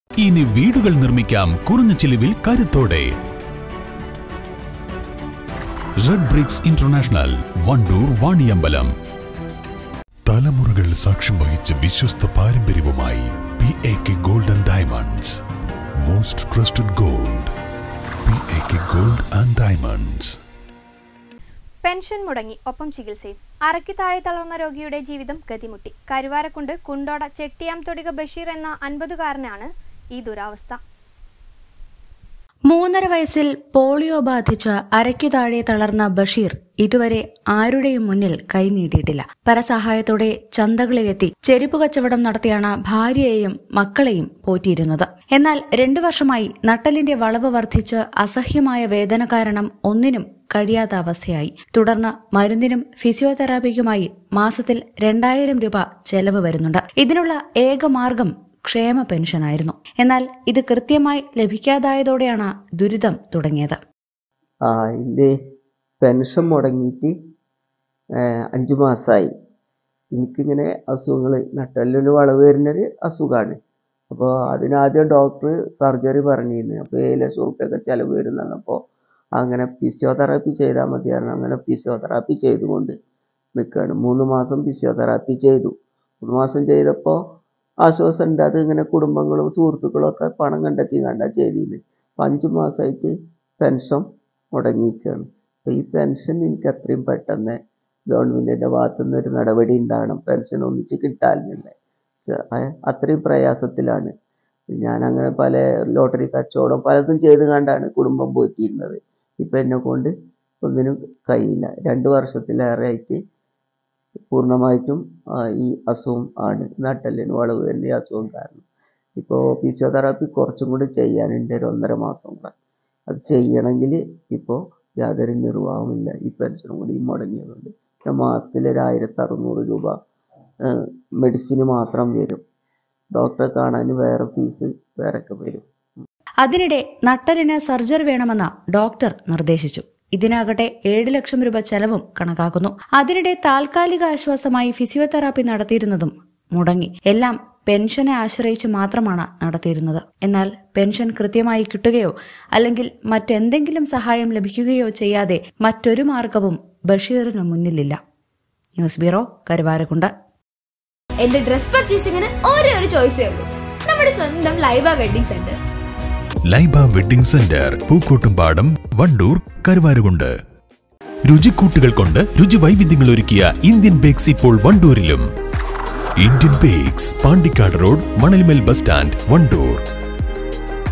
പെൻഷൻ മുടങ്ങി,ഒപ്പം ചികിത്സയും. അരക്കുതാഴെ തളർന്ന രോഗിയുടെ ജീവിതം ഗതിമുട്ടി|KARUVARAKUND
പെൻഷൻ മുടങ്ങി,ഒപ്പം ചികിത്സയും. അരക്കുതാഴെ തളർന്ന രോഗിയുടെ ജീവിതം ഗതിമുട്ടി.